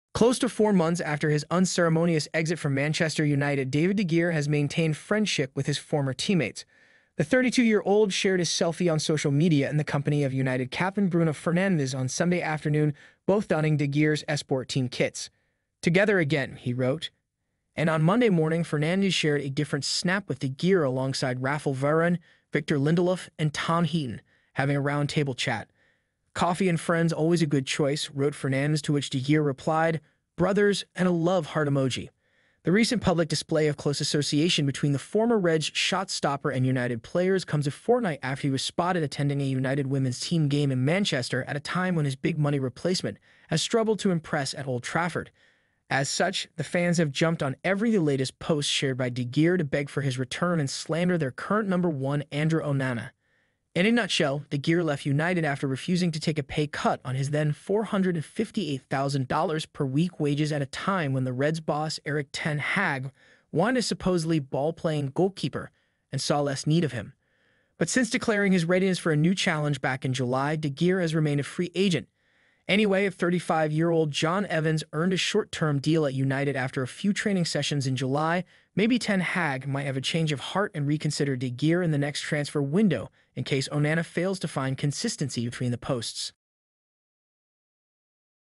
David de Gea Hangs Out With Bruno Fernandes & Man Utd Players Amid Andre Onana's Struggles
Former Manchester United goalkeeper continues to maintain a close association with former teammates almost 4 months after his controversial departure.